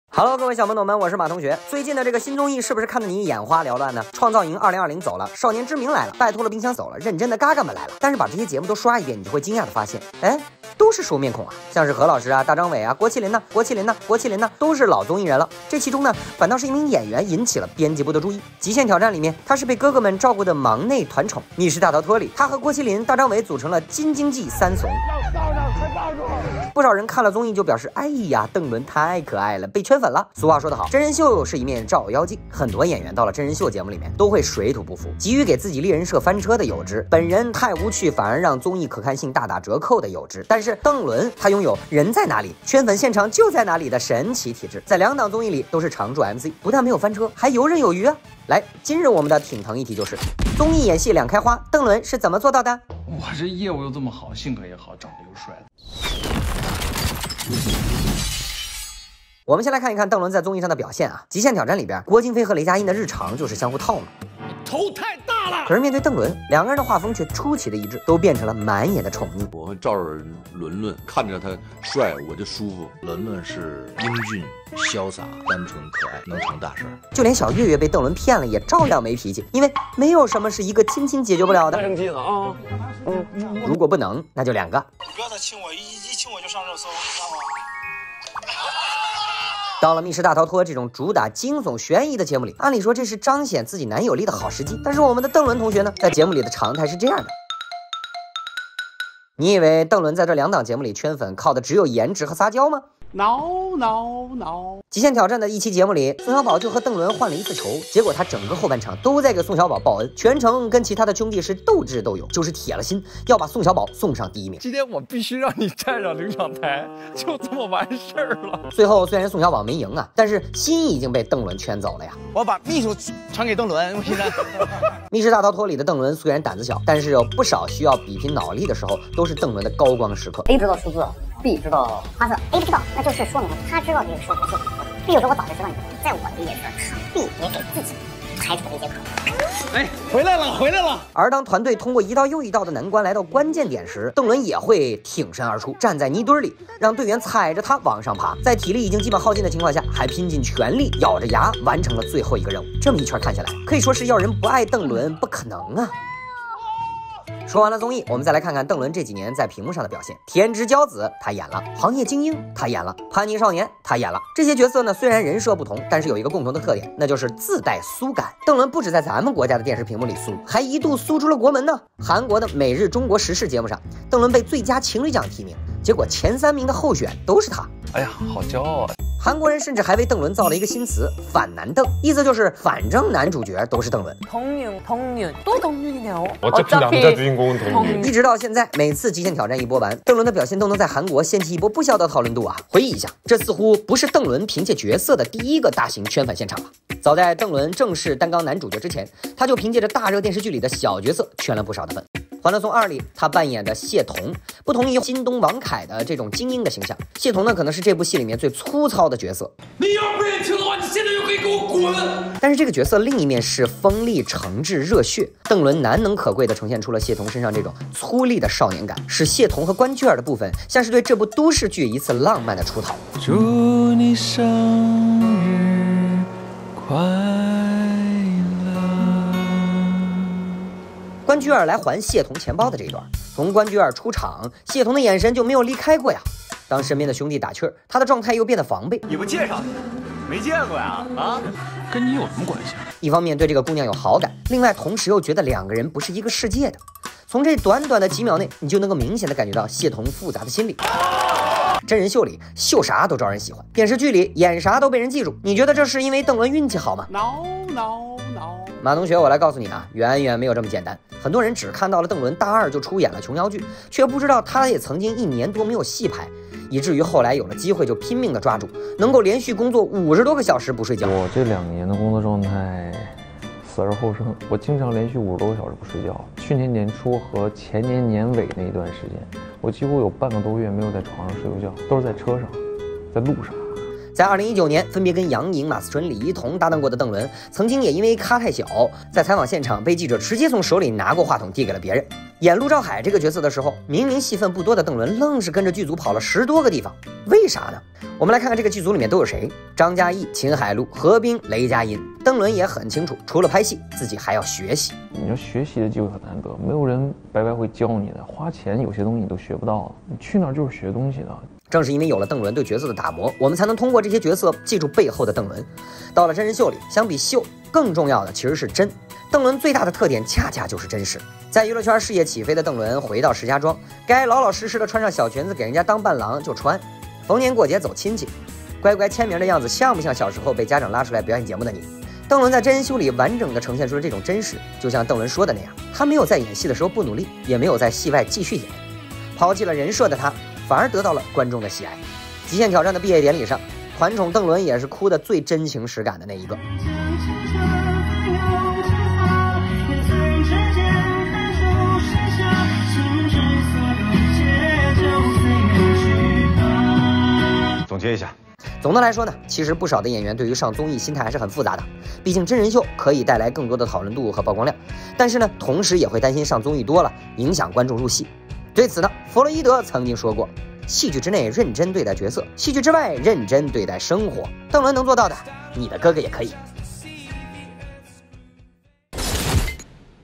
【理娱打挺疼】撒娇男人最好命？来看邓伦在甜和苏之间自由切换！
理娱打挺疼 不是每一种牛奶都叫邓伦苏！石家庄之光#邓伦 到了综艺里竟然变成了行走的撒娇精小甜豆？今天编辑部就带大家看看，从电视剧到综艺，凭实力圈粉的邓伦到底有啥过人之处.